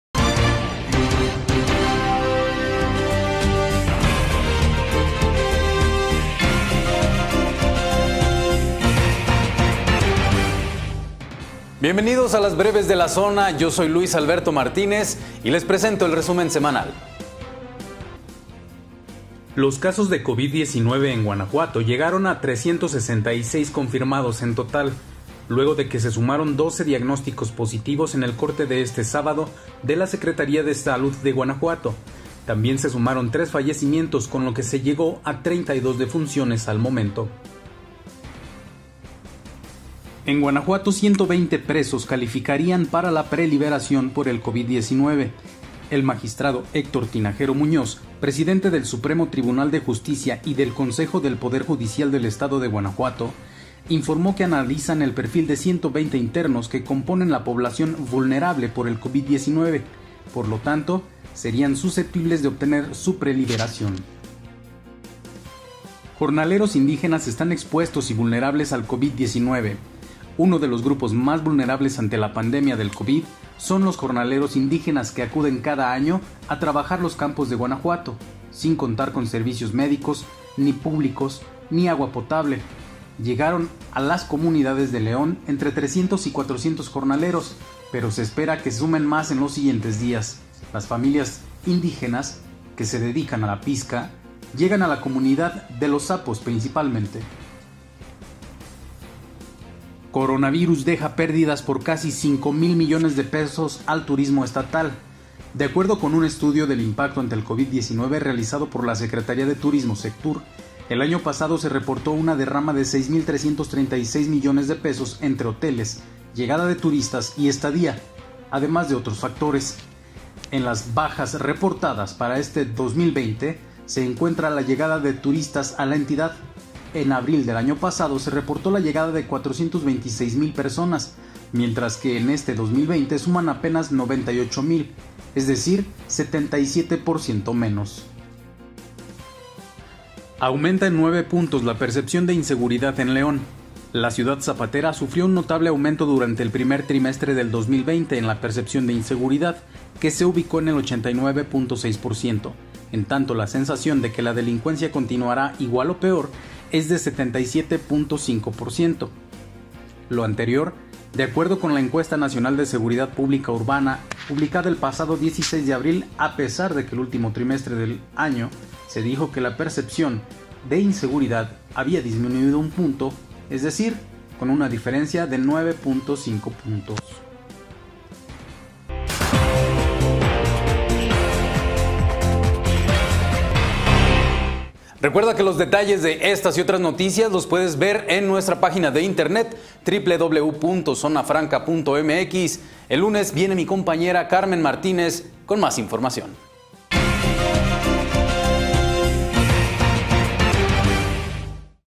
Casos de COVID-19 en Guanajuato llegan a 366 | Breves de la semana
BrevesDeLaZona📰 | Este es el resumen con las noticias más importantes ocurridas durante esta semana en Guanajuato.